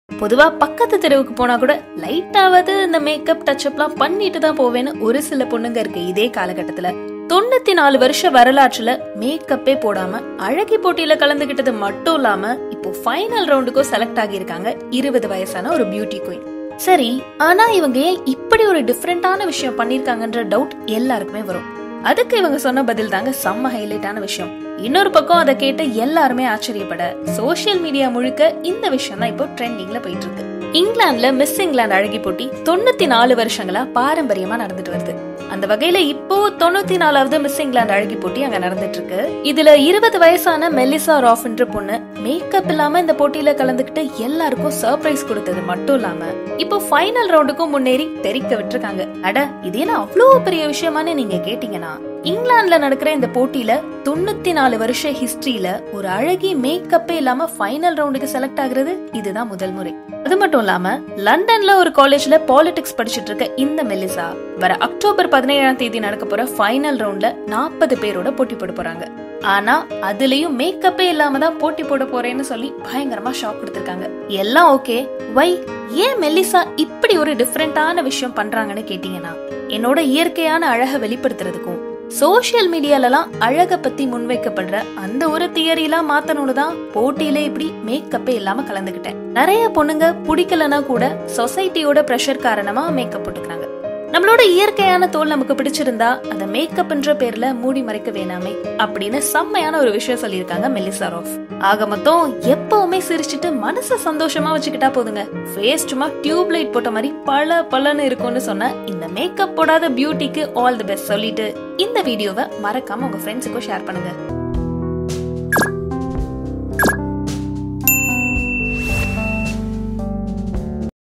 94 வருடங்களில் முதல் Makeup இல்லாத அழகி | இங்கிலாந்து போட்டியில் அசத்தும் பெண்
94 வருடங்களில் முதல் Makeup இல்லாத அழகி | இங்கிலாந்து போட்டியில் அசத்தும் பெண்MissEngland